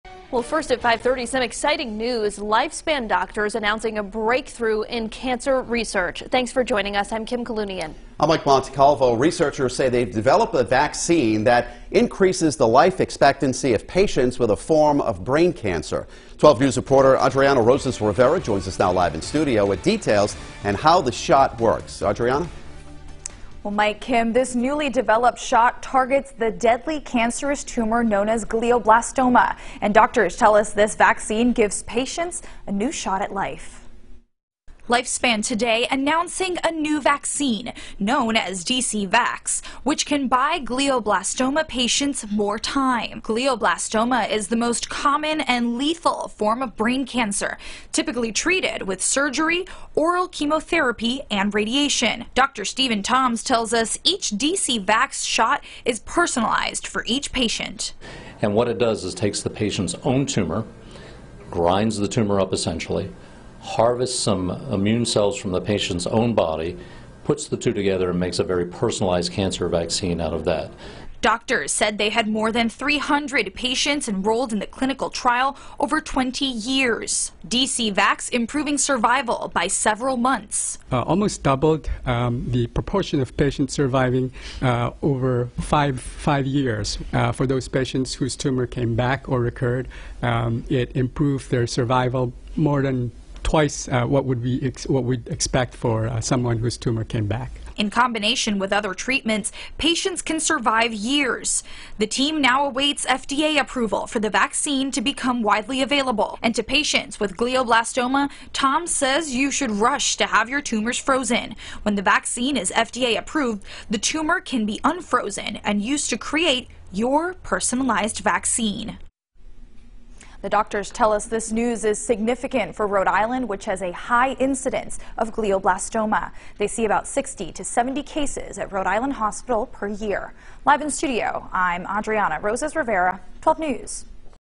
Researchers announce breakthrough in brain cancer vaccine trial
The Lifespan Cancer Institute held a news conference at Rhode Island Hospital to present results from a vaccine trial for glioblastoma, the most common and lethal form of brain cancer.